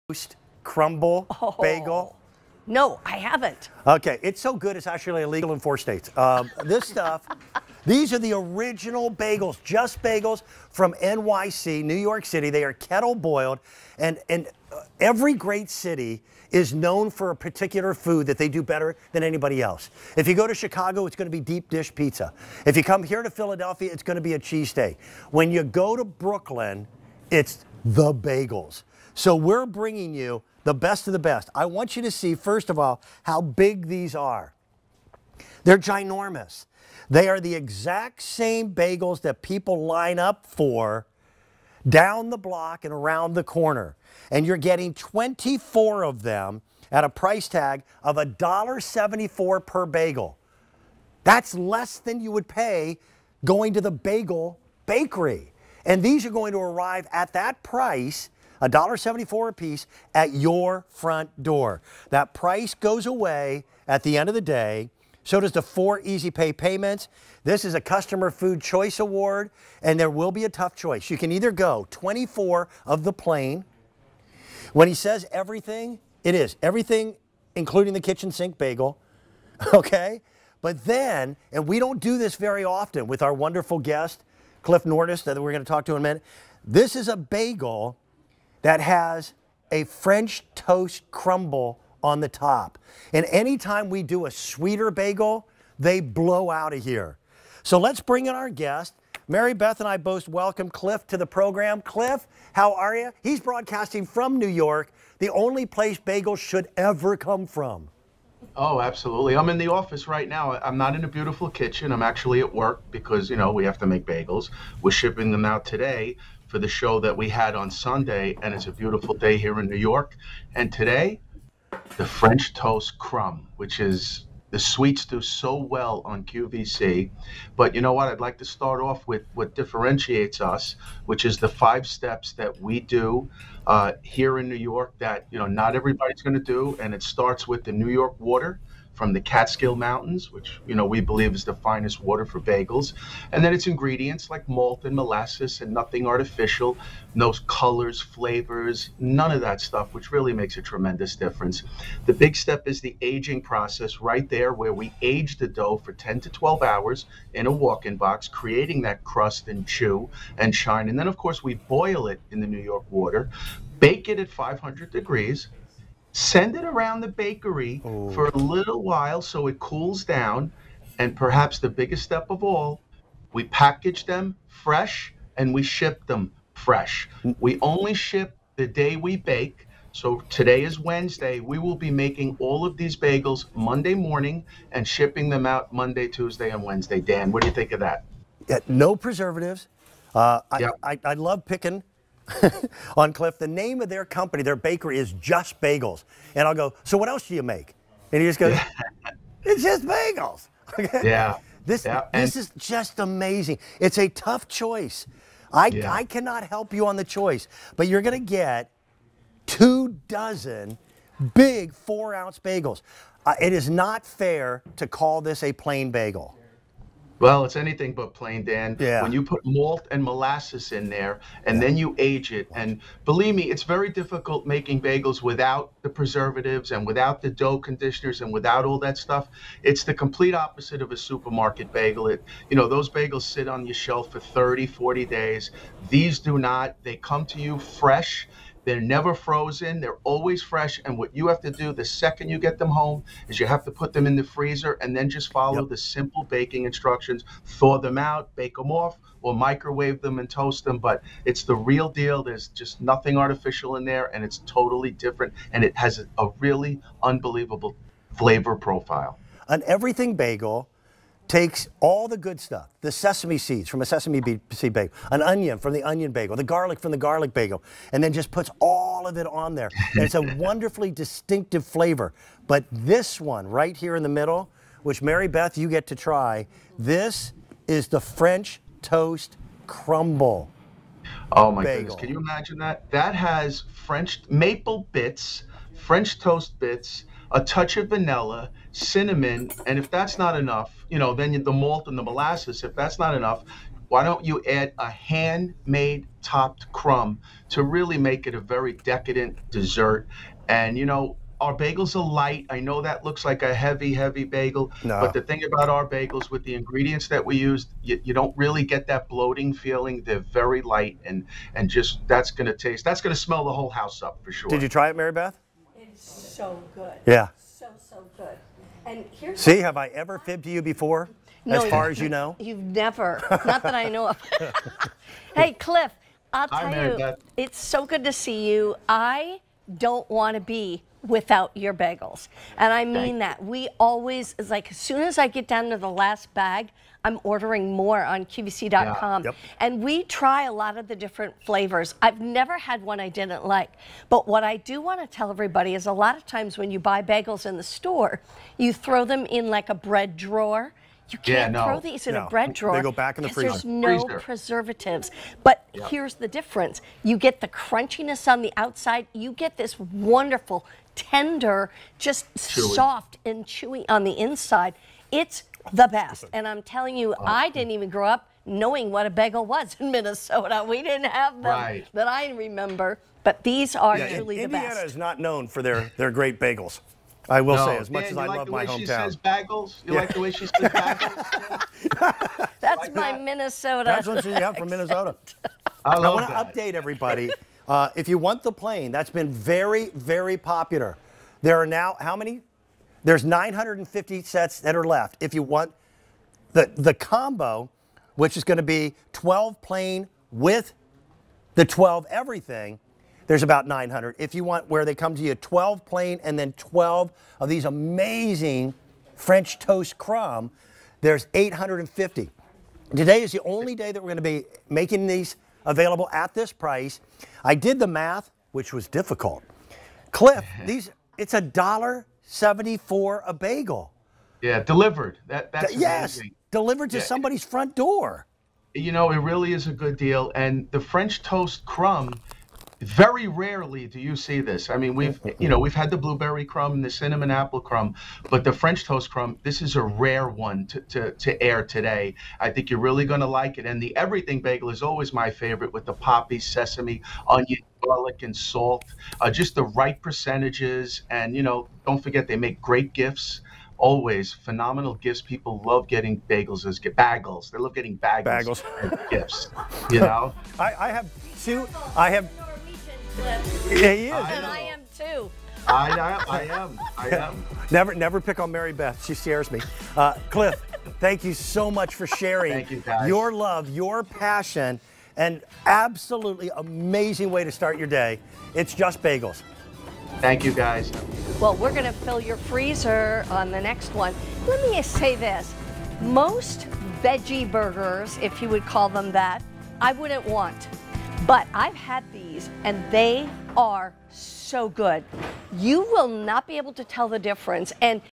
Just Bagels NYC Kettle Boiled (24) 4-oz. Bagel Sampler on QVC
Just Bagels NYC Kettle Boiled (24) 4-oz. Bagel Sampler

Get that chewy, doughy NYC bite with no train or plane involved -- these kettle boiled bagels bring authentic Big Apple flavor right to your breakfast table. Plus, they're pre-sliced and ready for smearing with your favorite spread. From Just Bagels.